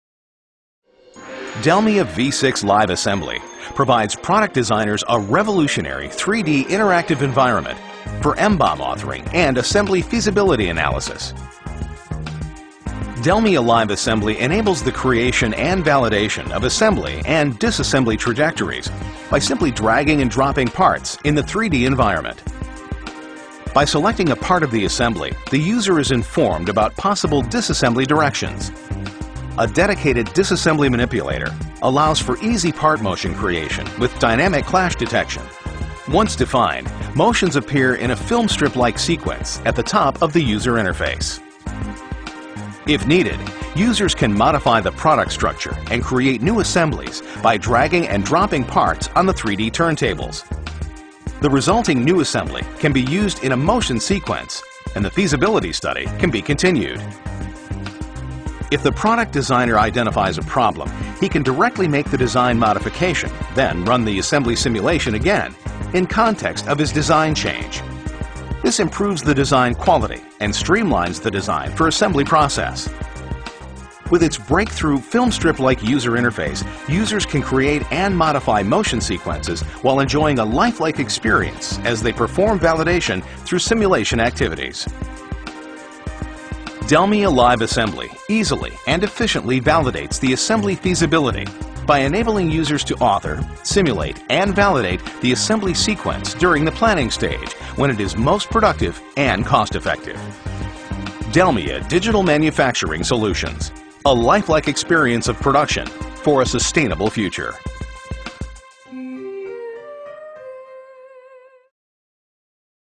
Montagesimulation & Arbeitsvorbereitung mit DELMIA
Mit den DELMIA-Lösungen von Dassault Systèmes können Sie jederzeit, schnell und effizient, Ihre Konstruktion auf die Montage- und Wartungsfreundlichkeit überprüfen. Sie vermeiden Änderungsschleifen und reduzieren die Kosten in der Anlaufphase.

Inhalt des englischen Videos:
The DELMIA LIVE Assembly video captures how the solution easily and efficiently validates the assembly feasibility by enabling users to author, simulate and validate the assembly sequence. Images by Courtesy of WOOK-IL Machinery Co. LtdA DELMIA executive eye catcher video showcasing the DELMIA V6 LIVE Assembly solution. Approximately two minutes in length, this professionally narrated and produced video, shows a great overview of key capabilities of this solution.

Sprechen Sie uns an:
Sales Center

Video: Mit freundlicher Unterstützung von Dassault Systèmes